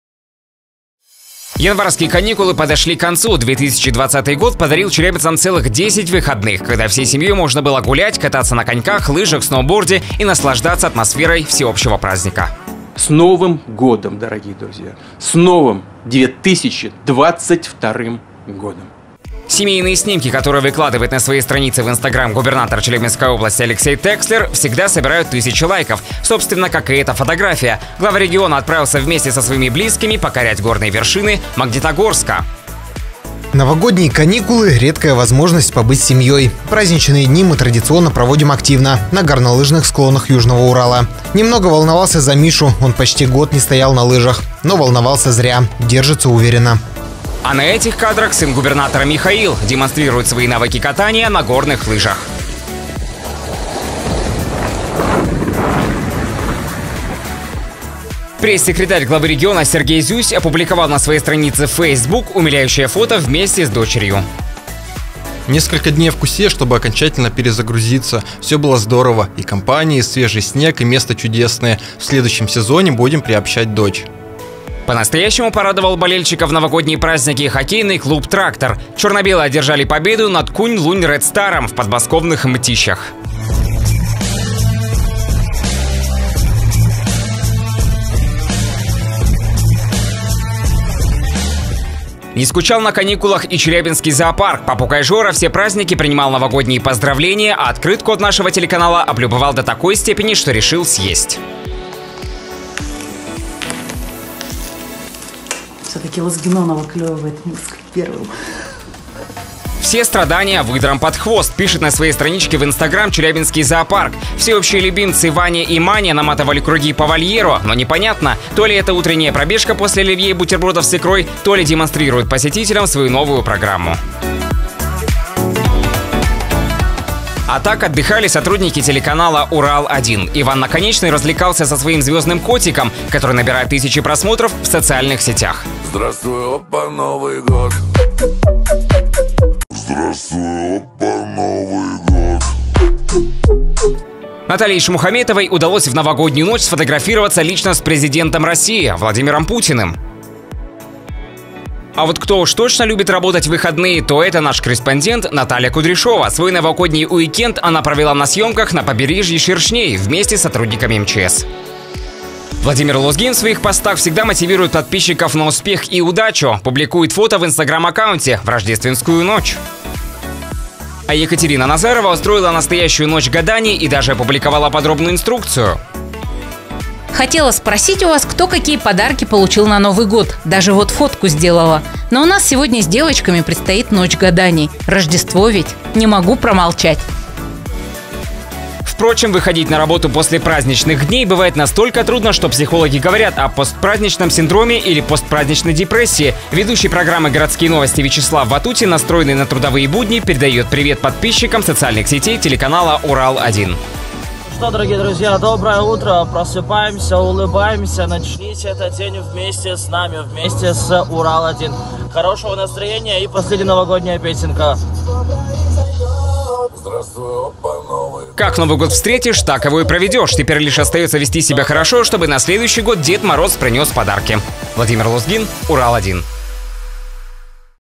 Катание на горных лыжах, гадание в ночь перед Рождеством, поздравление президента,гонки на снегоходе
Январские праздники закончились, а воспоминания о них остались!